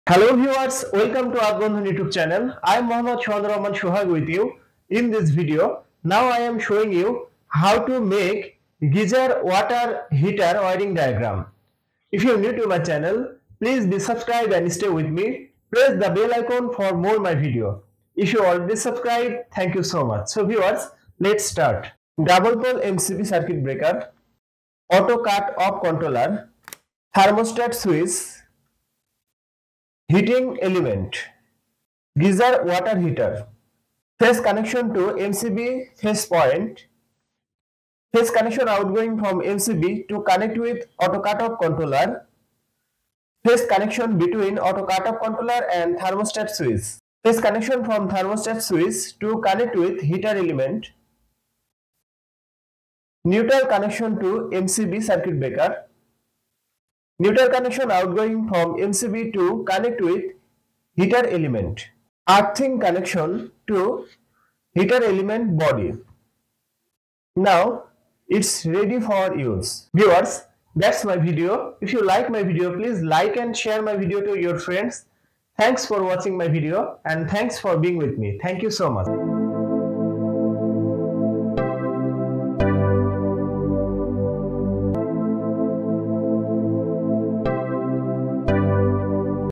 How to Make Geyser Water Heater Wiring Diagram
This video shows Geyser Water Heater Wiring Diagram. The Geyser Wiring works is simply the conversion of electrical energy into heat through the use of heating elements to raise the temperature of water through conduction of the heat to the water.